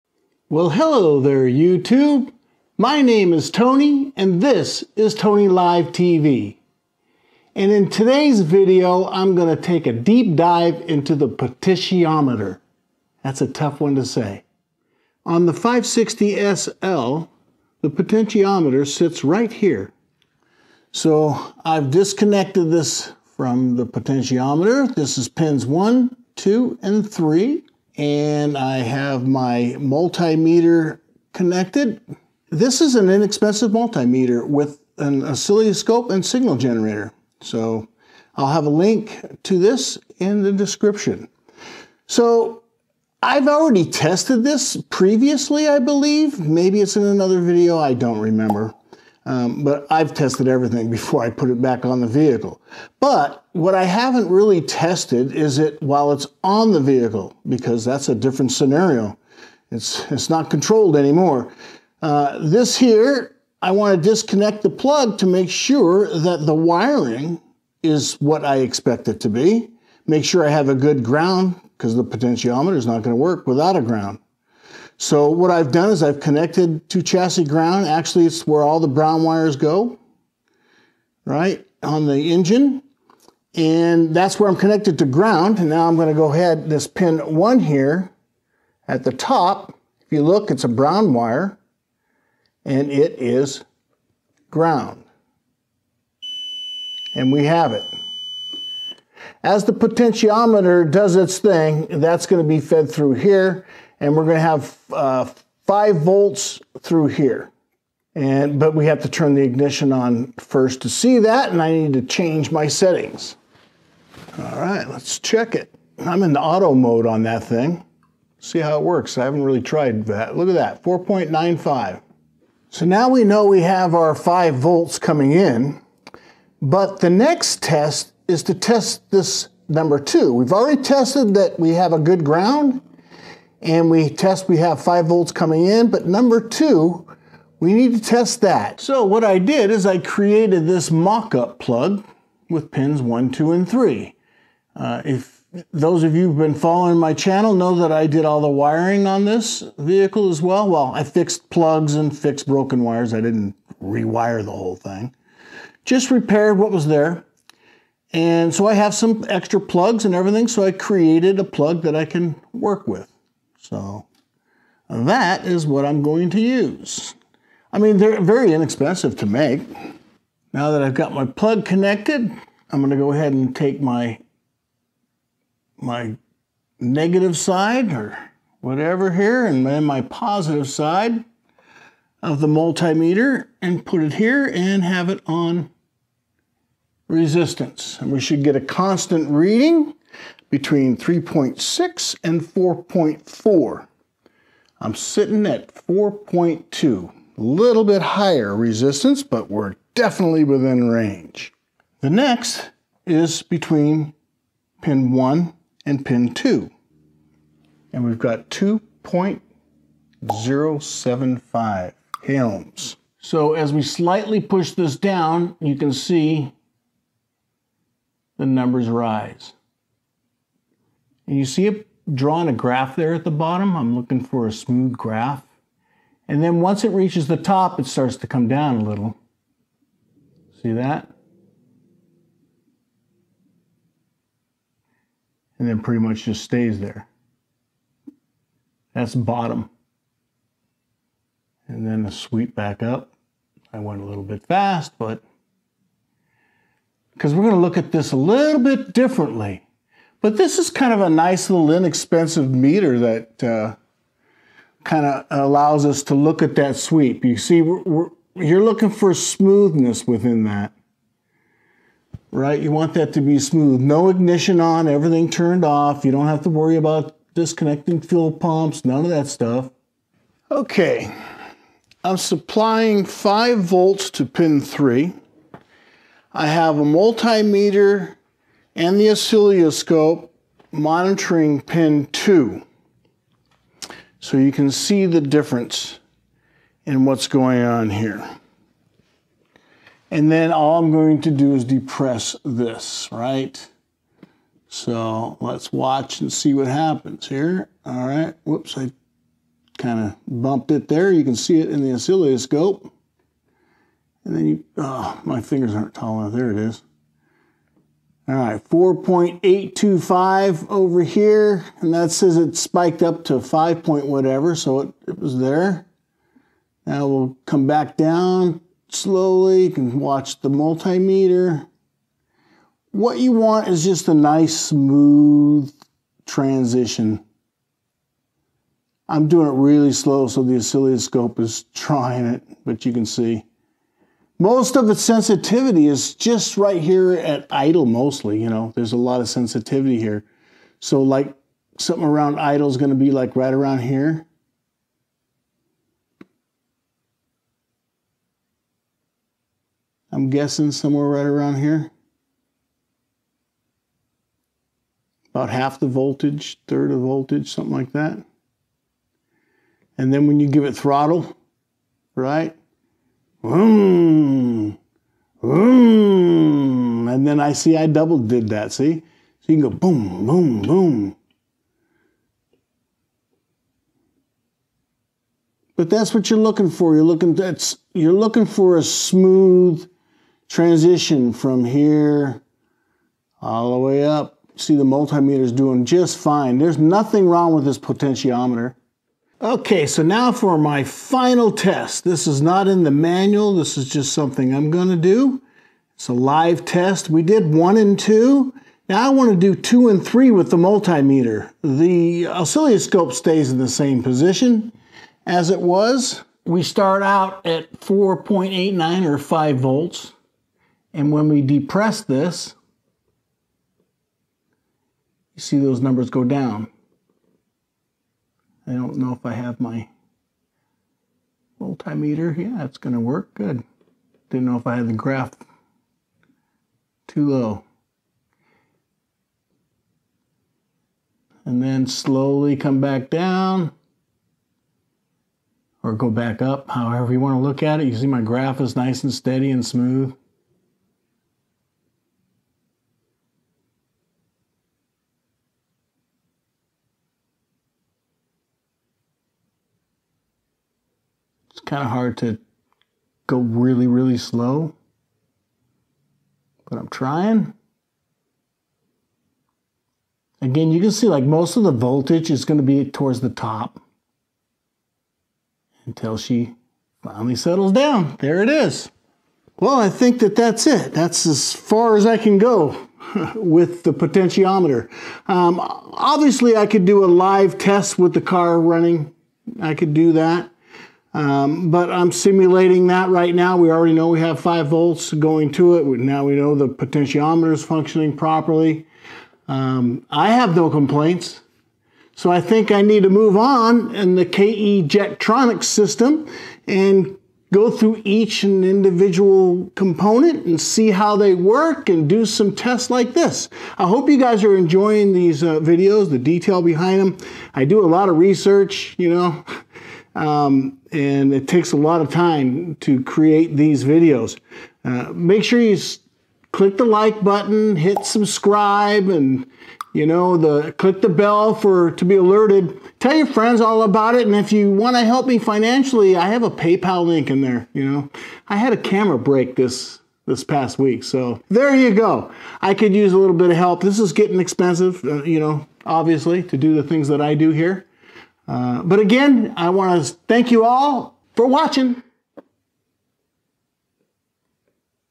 560SL - Potentiometer Explained in Under 10 Minutes!
🚗 Mercedes-Benz 560SL Potentiometer: Quick & Complete Testing Guide! 🚗

In under 10 minutes, I break down how to test and diagnose the airflow sensor plate potentiometer on my 1987 Mercedes 560SL. If you’re troubleshooting KE-Jetronic fuel mixture issues, this video gives you fast, real-world results without unnecessary fluff!

🔧 In this quick guide, you’ll learn:
✅ How to test the potentiometer with a multimeter and an oscilloscope
✅ Correct voltage & resistance values
✅ Why it affects fuel mixture
✅ Simple troubleshooting tips

The multimeter I have I purchased years ago, but this one has about the same specs:

The Oscilloscope I purchased is the FNRSI 2C53P. It works just fine, but I would like a better scope. This one I could afford. I got a smoking deal when I purchased it.